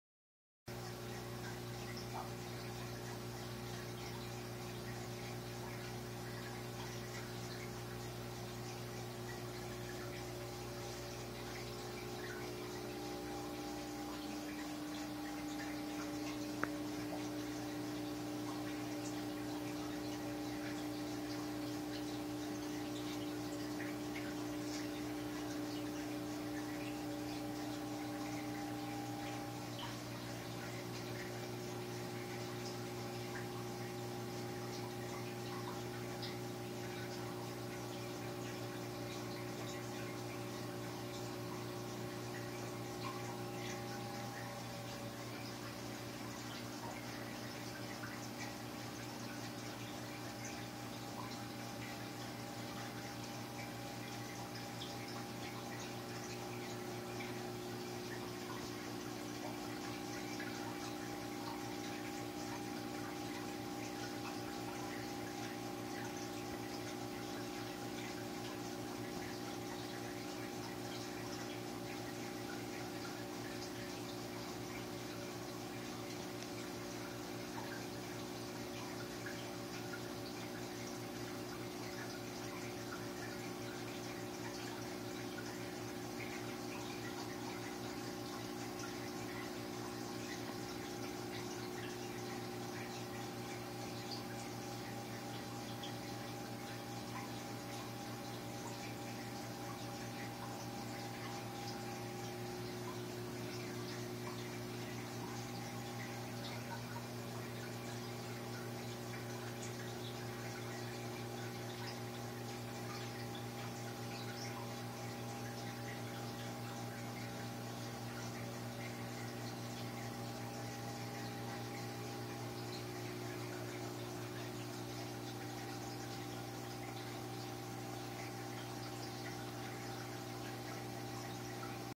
Cichlids fish tank with 3d background
6 f1 Zaire mobas and some peacock cichlids.